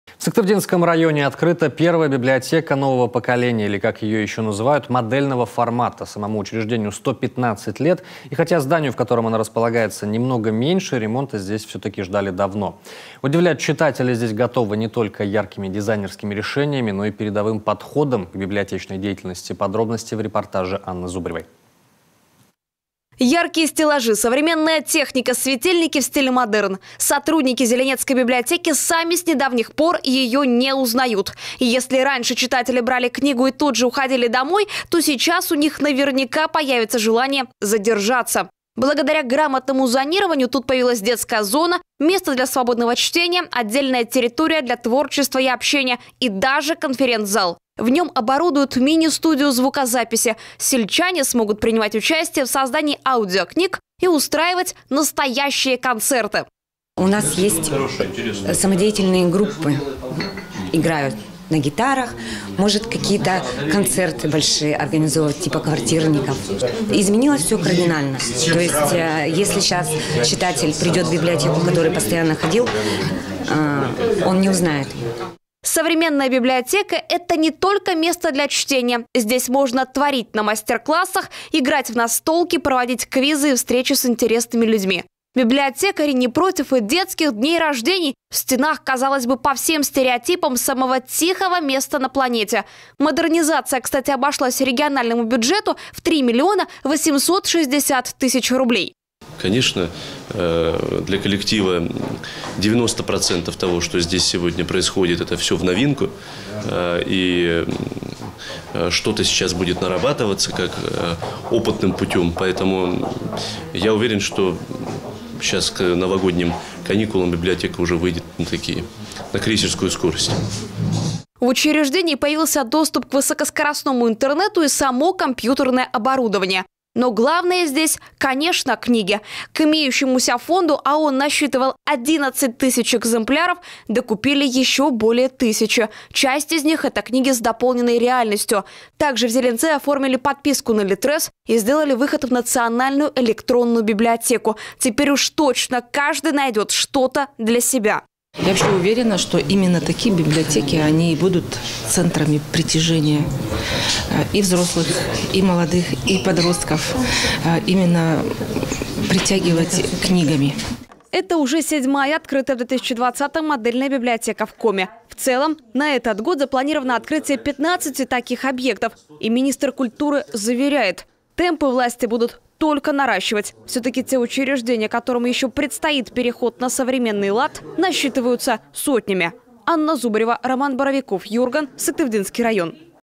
В Сыктывдинском районе открыта первая библиотека нового поколения
Или как её ещё называют «модельного формата». Самому учреждению 115 лет, и хотя зданию, в котором она располагается, немного меньше, ремонта здесь всё-таки ждали давно. Удивлять читателя здесь готовы не только яркими дизайнерскими решениями, но и передовым подходом к библиотечной деятельности.